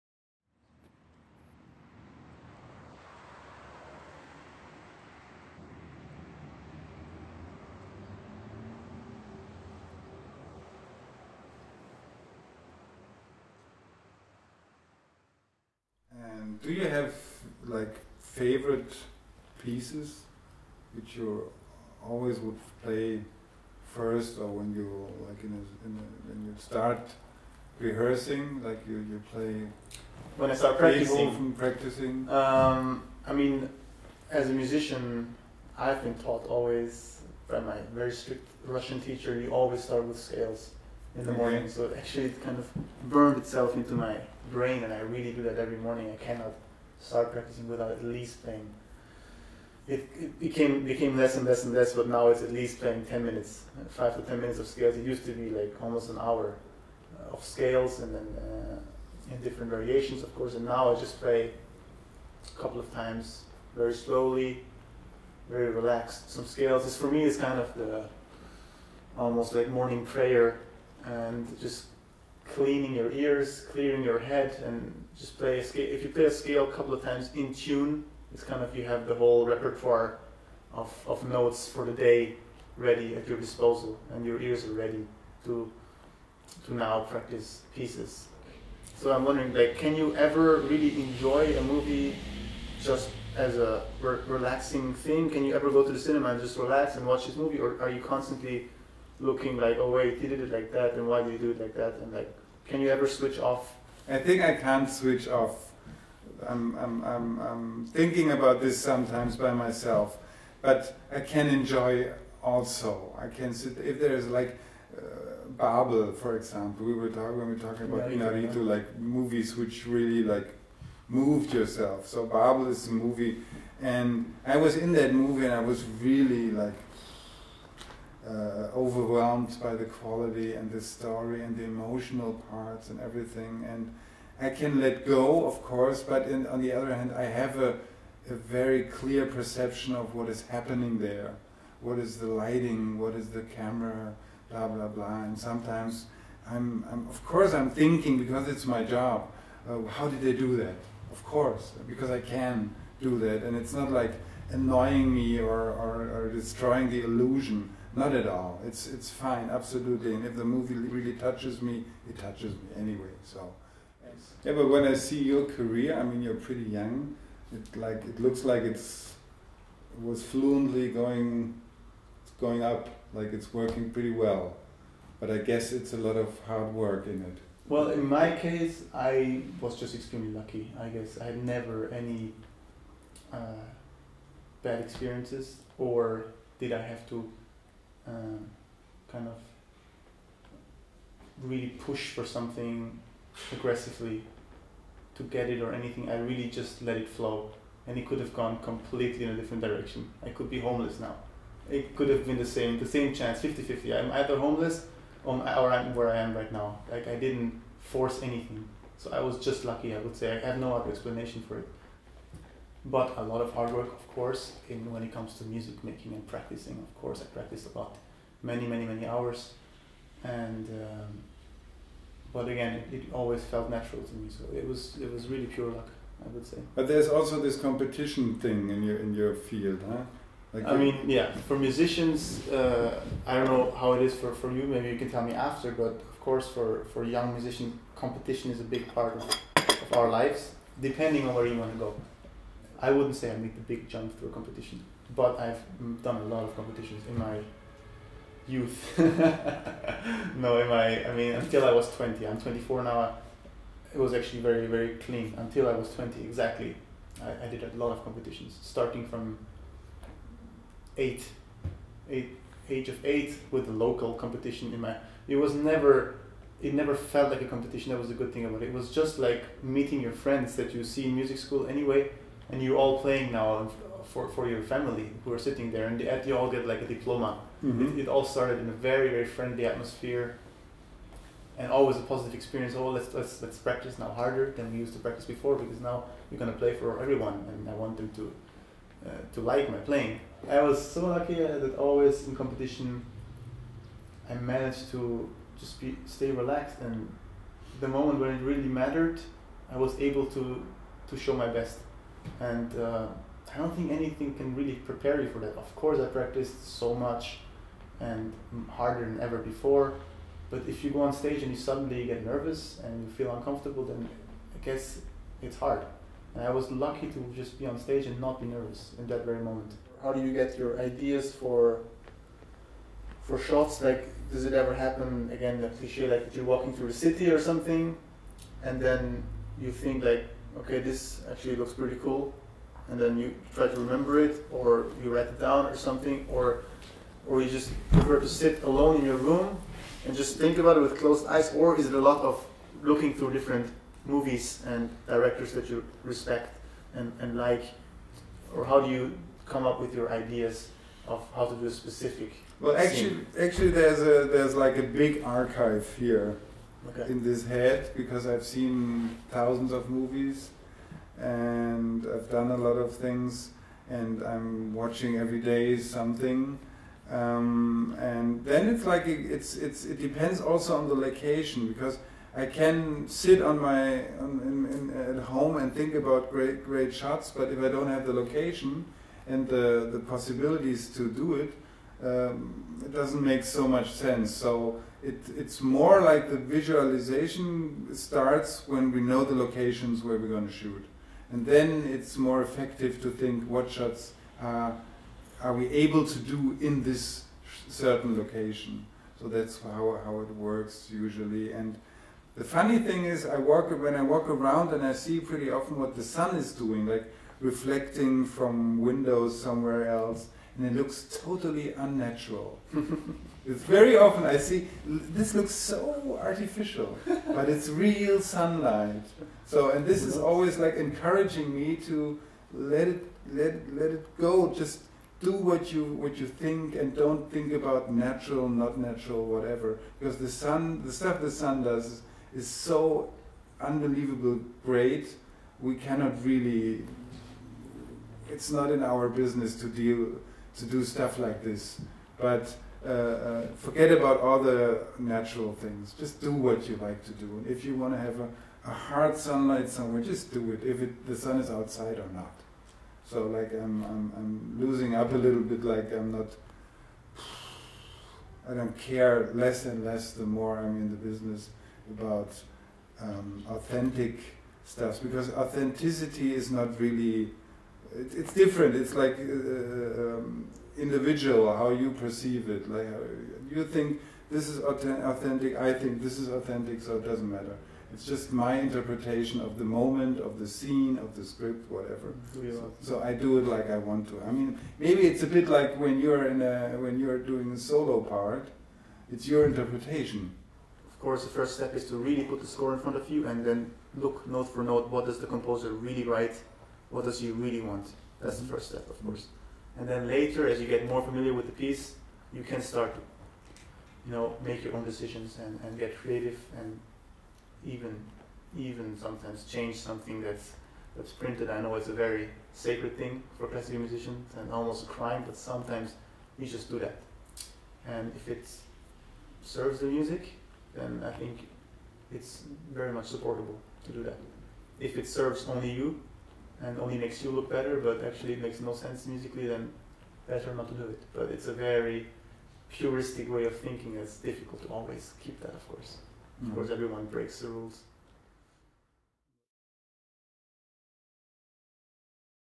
We Need to Talk | Kian Soltani and Gunnar Fuss
International soloist, Persian-Austrian cellist Kian Soltani told us about his fascination with cinema and film-making, so we hooked him up for a conversation with acclaimed director of photography, Gunnar Fuss. A cross-disciplinary talk took place at the lovely showroom of Central Berlin.